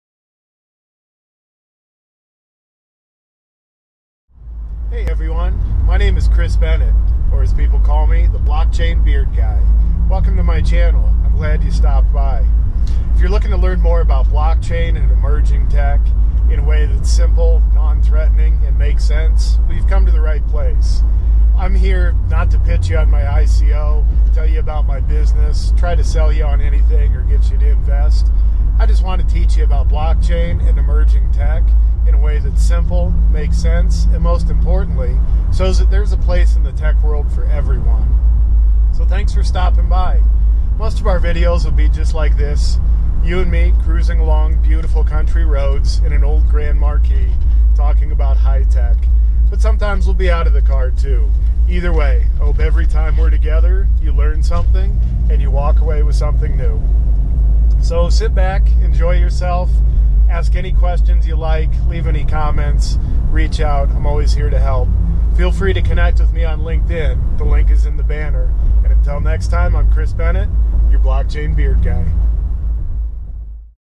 Blockchain Beard Guy Channel Trailer
Hi! I'm Kris Bennett, better known as the Blockchain Beard Guy. I come a from a 20+ year background in custom solutions design and delivery. I'm on a mission to educate the world about blockchain and emerging technologies.

I started this channel to spread knowledge and to show EVERYONE that they have a place in technology, regardless of personal or professional background or perceived ability.

Let me know how I'm doing, and let me know any topics you'd like me to cover, and most importantly, thanks for riding along! :)